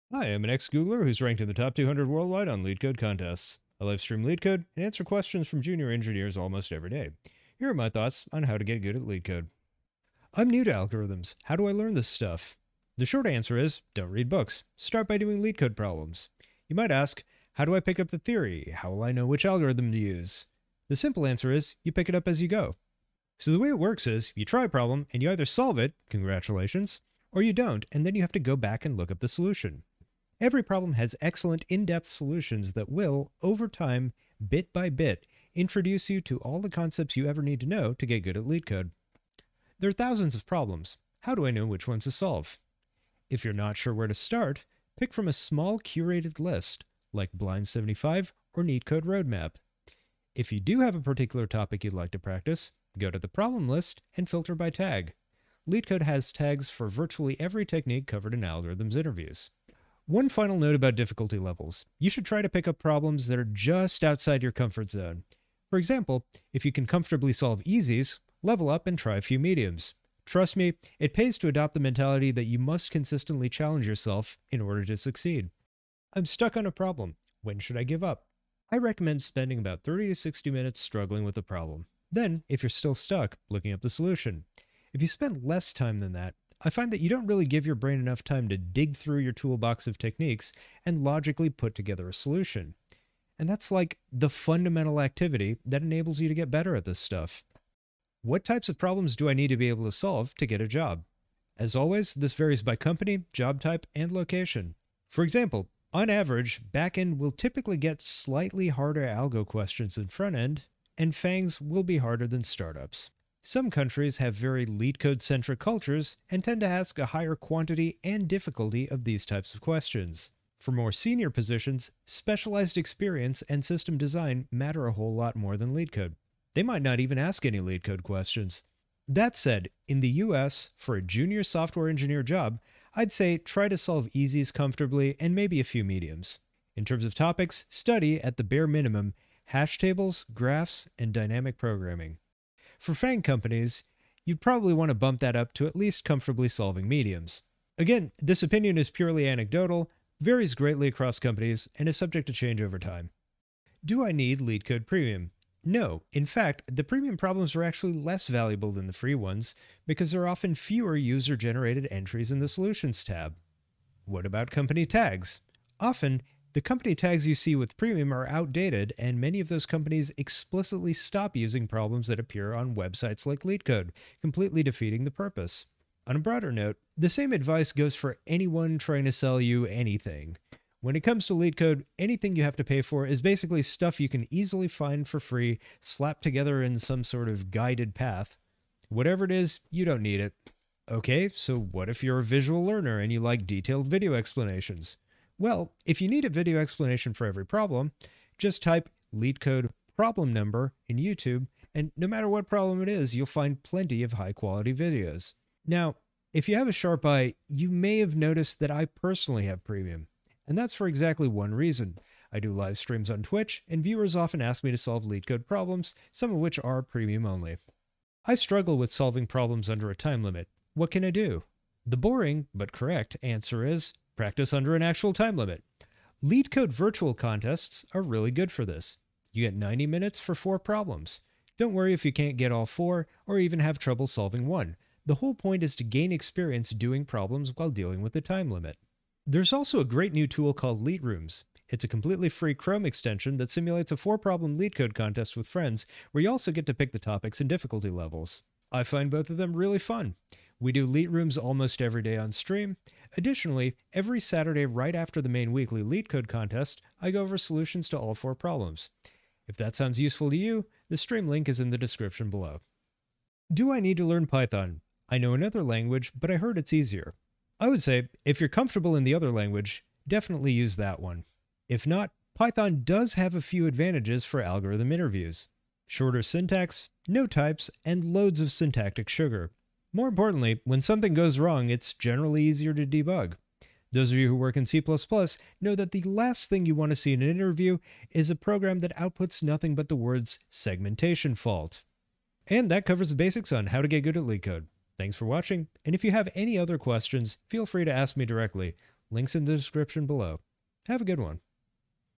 How To Get Good At LeetCode
An Ex-Googler and Top Ranked LeetCoder explains how to get good at LeetCode.

Timestamps:
00:00 I'm new to algorithms. How do I learn this stuff?
00:39 There are thousands of problems. How do I know which ones to solve?
01:16 I'm stuck on a problem. When should I give up?
01:38 What types of problems do I need to be able to solve to get a job?
02:33 Do I need LeetCode Premium?
03:38 I struggle with solving problems under a time limit. What can I do?
04:23 Do I need to learn Python? I know another language but I heard it's easier.